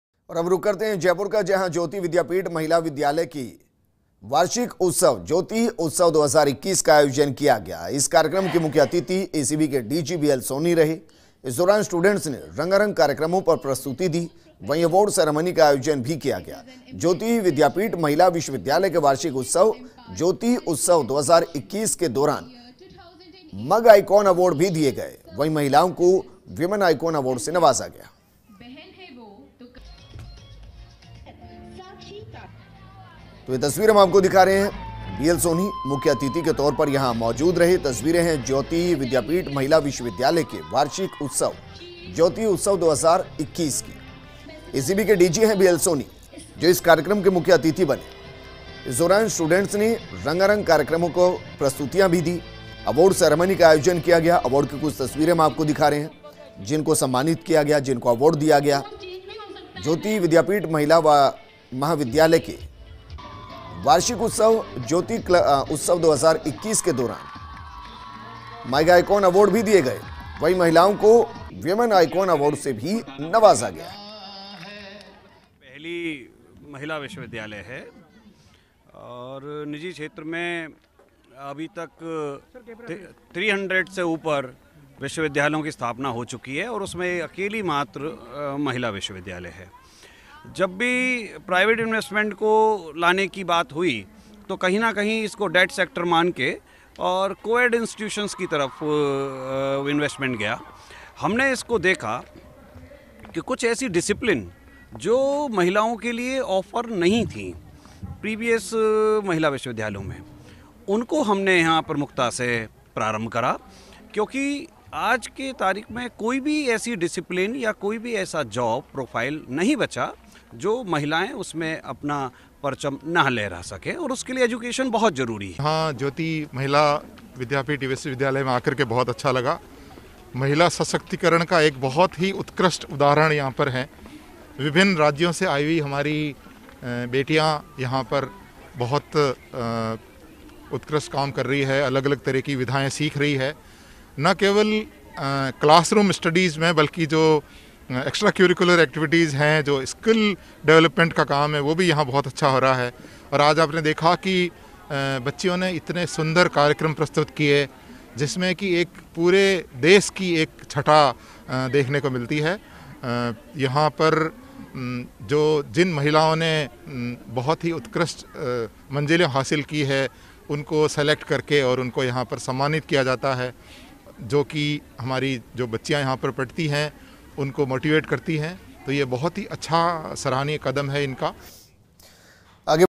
Jayoti Vidyapeeth Women’s University का वार्षिक उत्सव, महिलाओं को Woman icon award से नवाजा
Jayoti Vidyapeeth Women’s University का वार्षिक उत्सव, महिलाओं को Woman icon award से नवाजा FirstIndiaNews​ JayotiVidyapeethWomen​’sUniversity